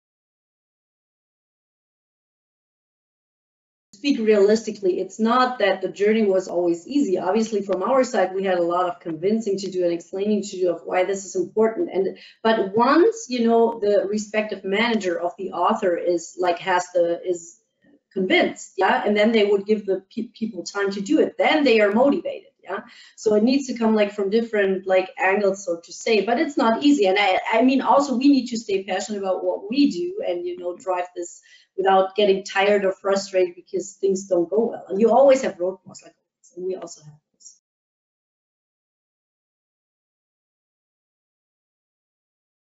How did the HR initiative at KIRCHHOFF motivate functional leads and authors to create content?
How did the HR initiative at KIRCHHOFF Automotive manage to get functional leads and authors to invest time in the project?

View full interview with Lisa Kitterer, Global Director Employee Development, Mary Sinischo, Employee Development Manager, KIRCHHOFF Automotive